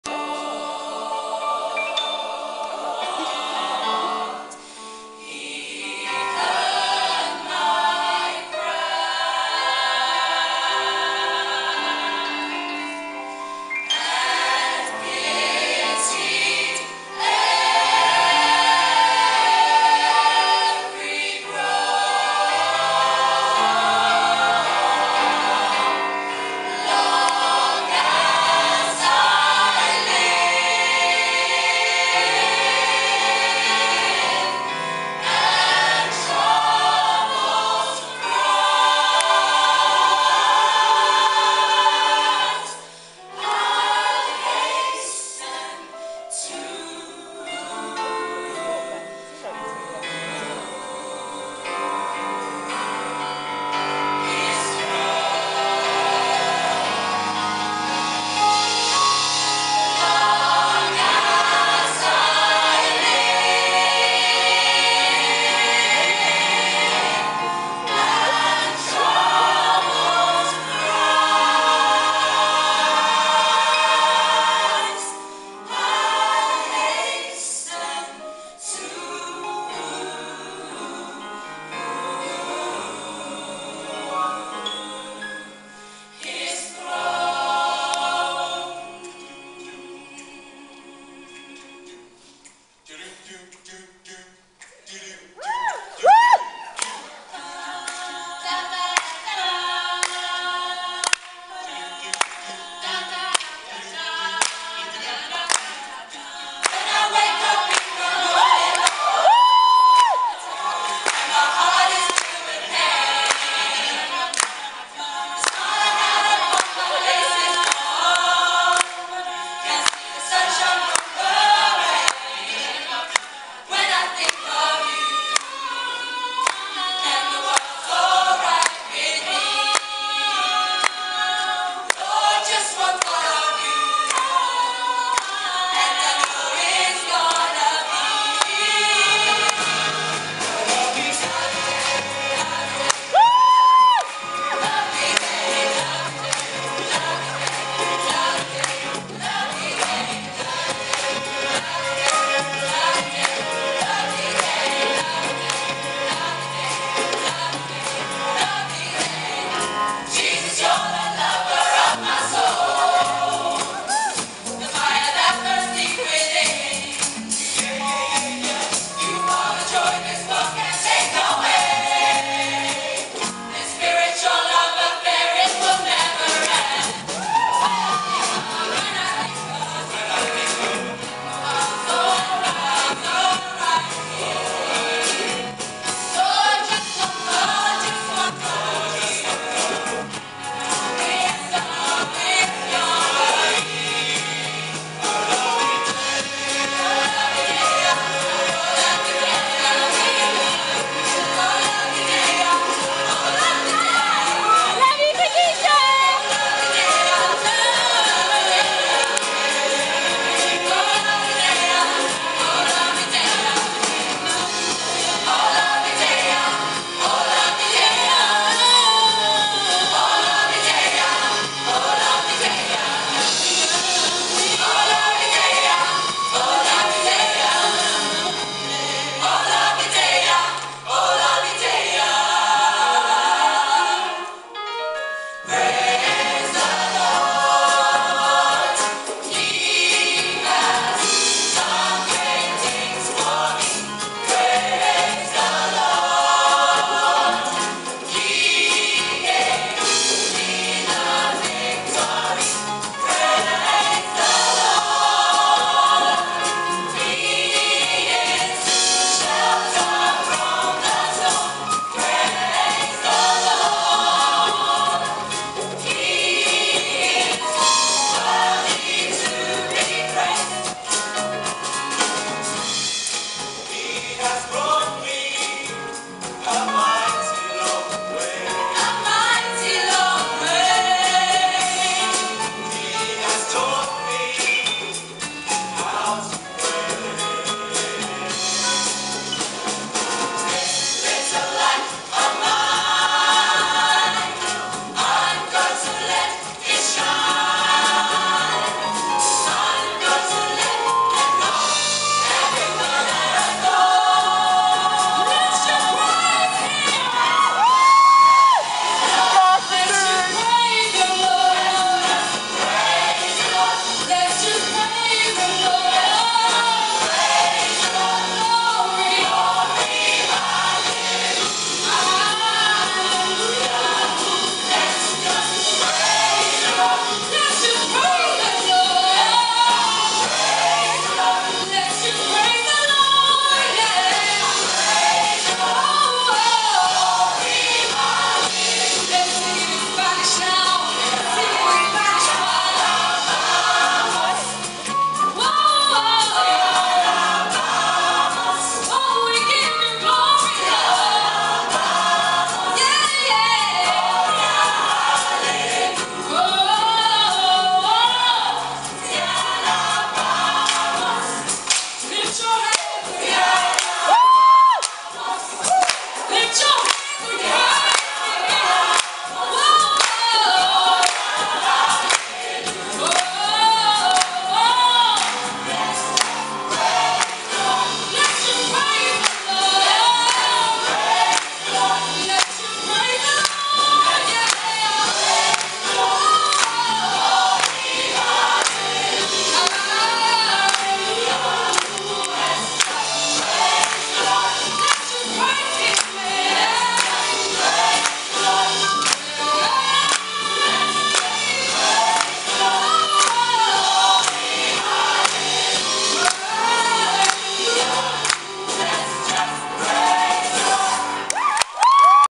Imperial College Gospel Choir @University Gospel Choir of the Year (UGCY) 2011
Imperial College Gospel Choir Perform University Gospel Choir of the Year 2011